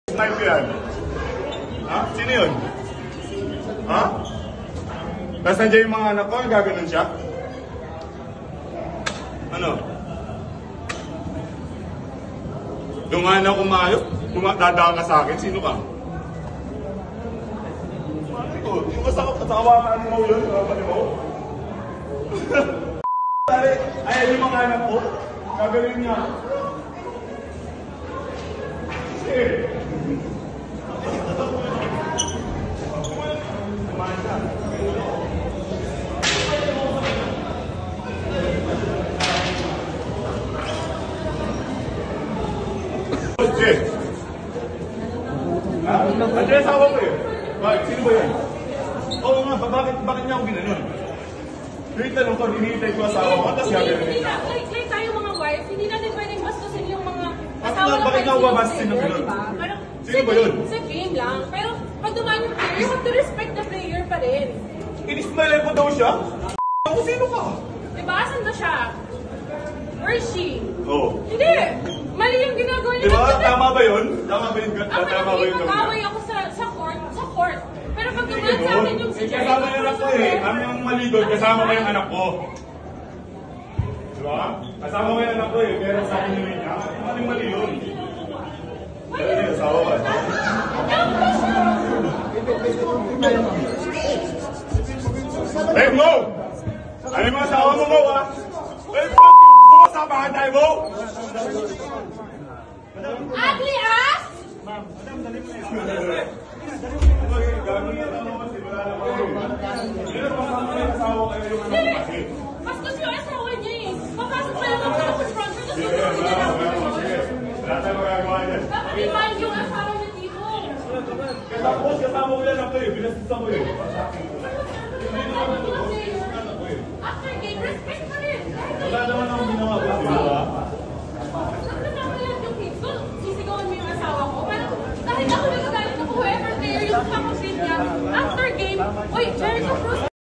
Calvin Abueva at asawa ni Mo Tautuaa, nagkainitan matapos ang game 2 ng PBA finals
Nagkasagutan sina Magnolia Hotshots forward CalvinAbueva at asawa ni San Miguel Beermen center MoTautuaa na si Aida matapos ang Game 2 ng PBA Commissioner’s Cup finals series, Feb. 4. Nakisama rin sa bangayan ang asawa ni Abueva.

Giit ni Abueva, binastos siya ni Aida. Buwelta naman ng asawa ni Tautuaa, si Abueva ang nambastos sa kanya habang naglalakad papunta sa banyo.